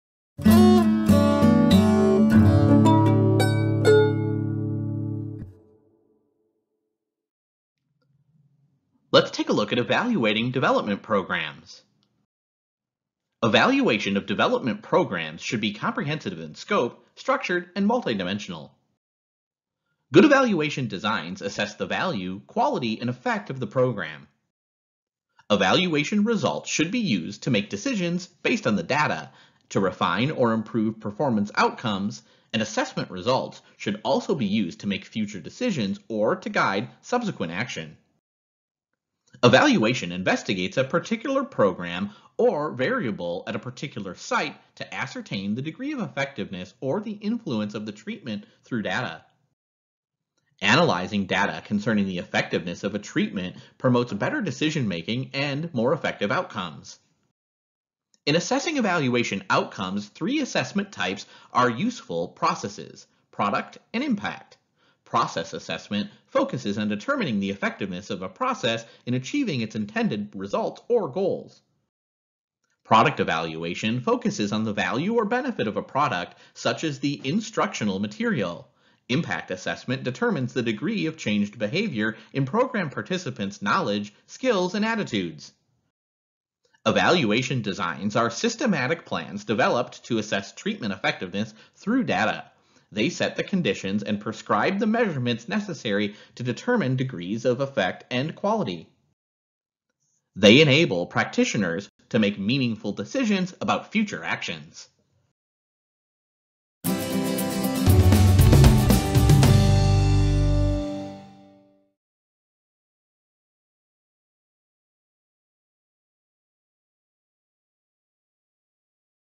Evaluating Development Programs
Maximizing human potential is accomplished through process-structured, custom-designed programs. Maximizing human capital comes not from control, but from liberating intelligence, from fostering pride in one's work, and from allowing people to have a vested interest in their accomplishments.

Traditional organizational structures work against the benefits of distributed intelligence-freely sharing knowledge and ideas and working cooperatively to improve continuously both individual and organizational goals. Information systems are needed that encourage the use of data and creativity to solve problems and make decisions.